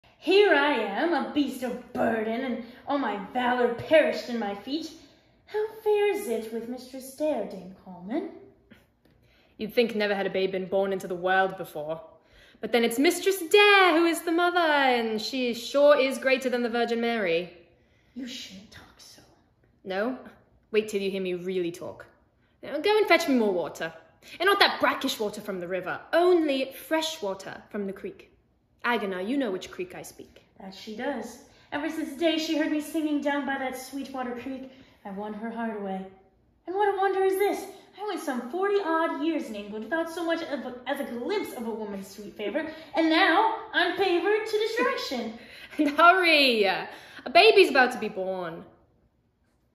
Jenna Moloney - Lost Colony sides
Dame Coleman sides for Female European Ensemble Submission